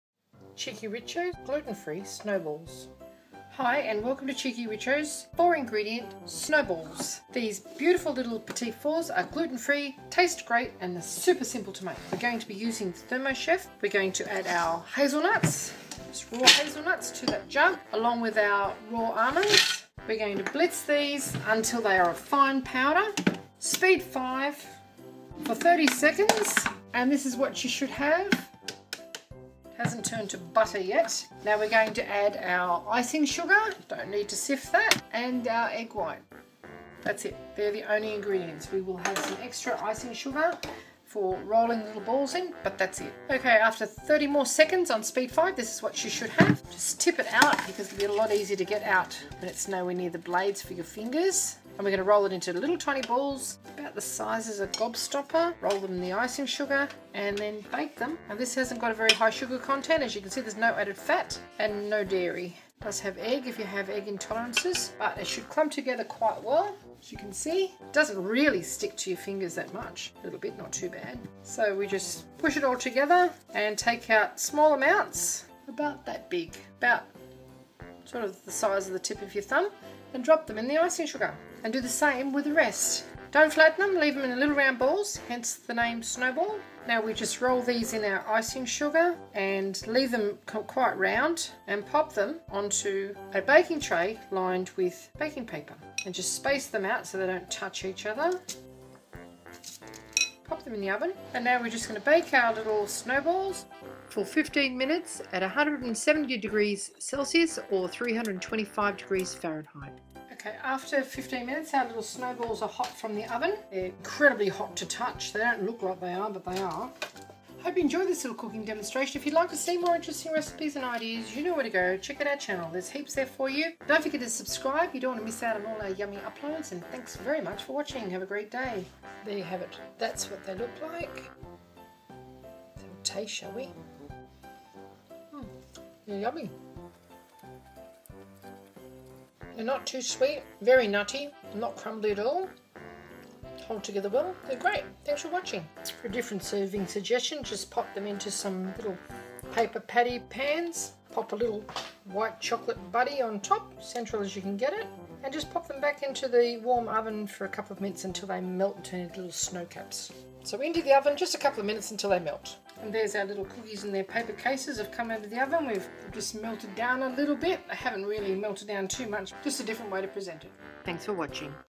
Snowballs Gluten Free 4 ingredient Thermochef tutorial cheekyricho
cheekyricho Snowballs or Boules de Neige. Using only 4 ingredients. They are gluten and dairy free, with no added fat and are not too sweet. This crisp little nutty treat makes perfect Petit Fours to have with coffee or cocktails. Make them in a minute and bake them in 15 minutes, they have to be the easiest little treats you can make at home for your holiday table. Do give them a try. Hope you like them as much as we do.

We used the Thermochef but if you don't have one use a blender to mill the nuts or even buy ground nuts for the recipe and mix by hand.
INGREDIENTS:

120g raw almonds
120g raw hazelnuts
1 egg white
80g icing sugar for the Snowball mixture  and about 80g more to roll them in.